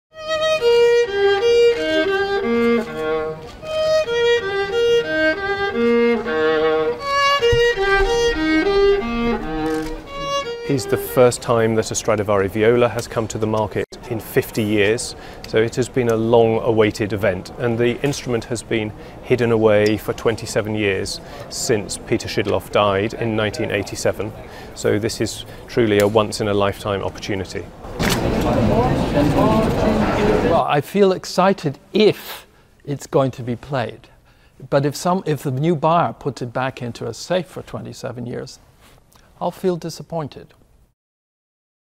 Rare Stradivarius viola expected to fetch $45M at auction
A rare 1719 Stradivarius viola is expected to sell for more than $45 million in a private sale by Sotheby's and Ingles & Hayday. The instrument is one of two violas by Antonio Stradivari still in private hands, with only 10 Stradivari violas known to have survived to the present day.